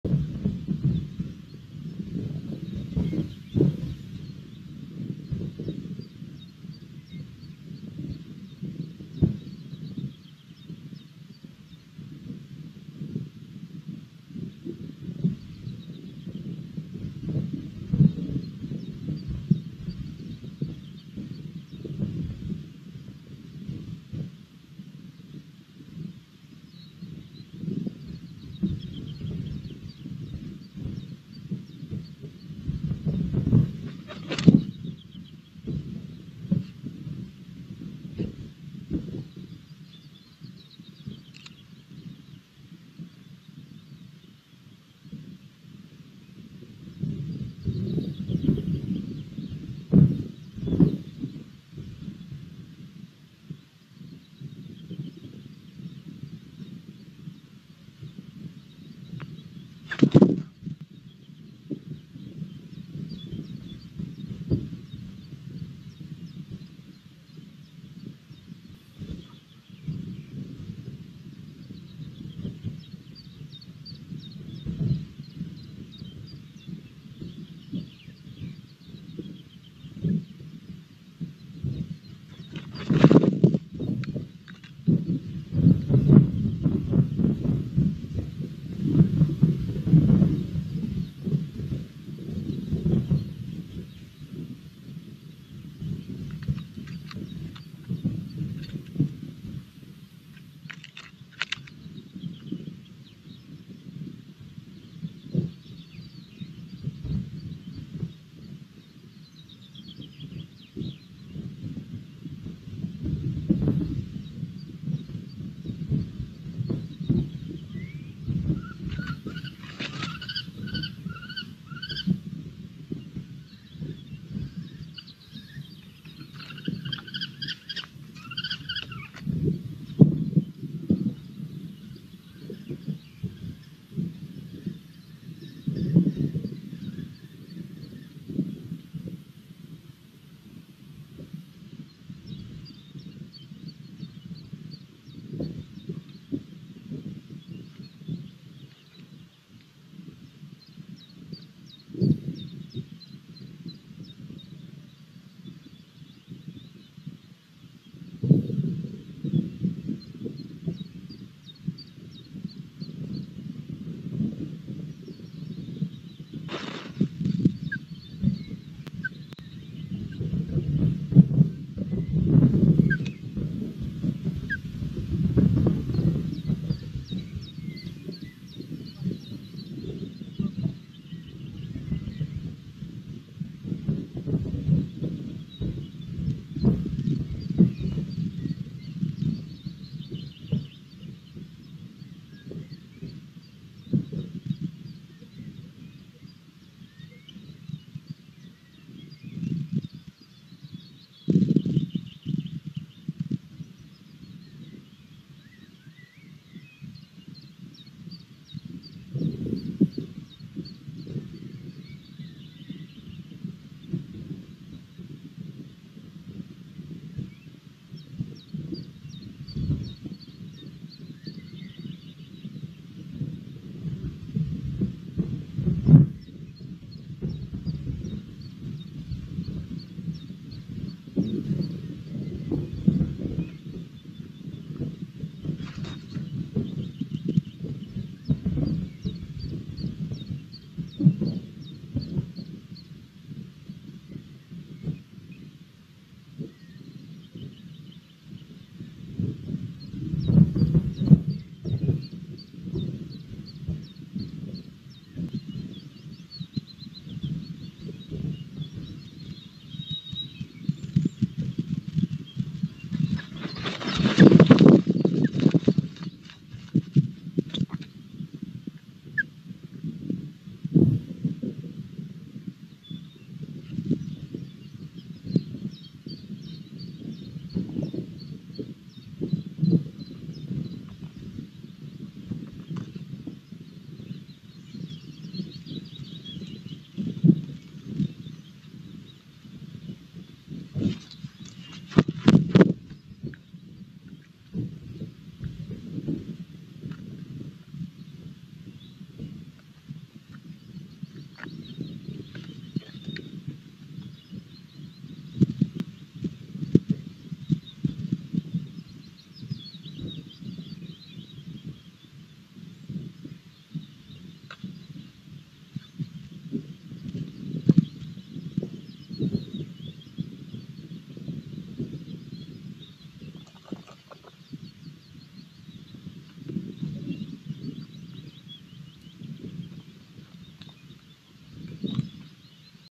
"We like the bark effect decor!" Polish Ospreys. 24 April 2023
Tip and Napi have serious Interior Design skills! :-)